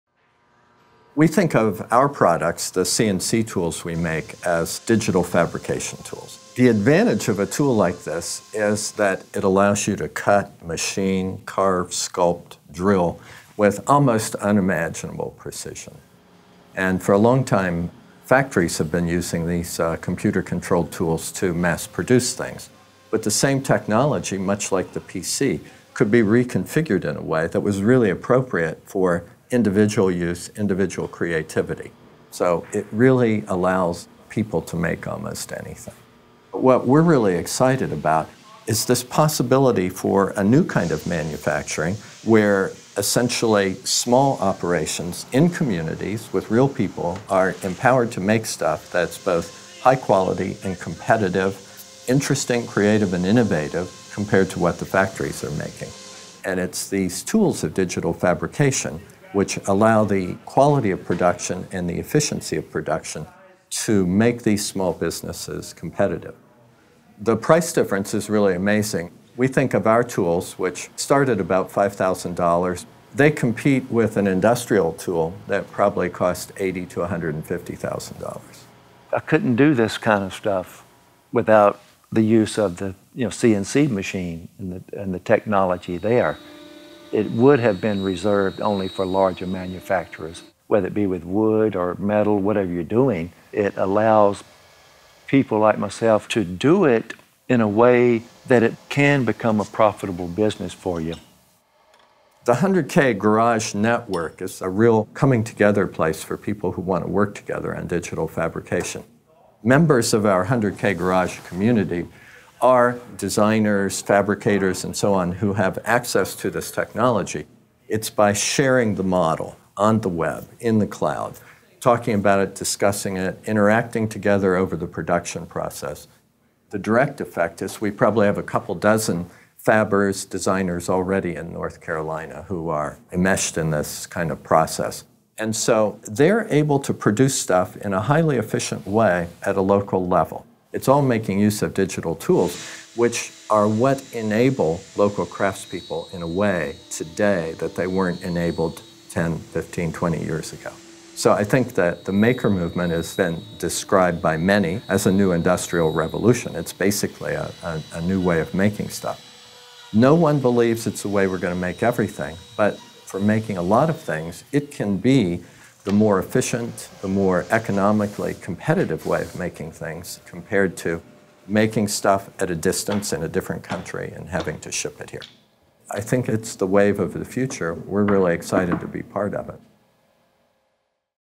Reimagining Traditional Manufacturing
The next industrial revolution is taking place in someone's garage. Known as the "maker movement," this more individualized, small-scale approach to manufacturing is helping spur locally networked communities. Today, North Carolina's community of designers and makers are utilizing next generation digital fabrication tools to make it easier and less costly to manufacture a product.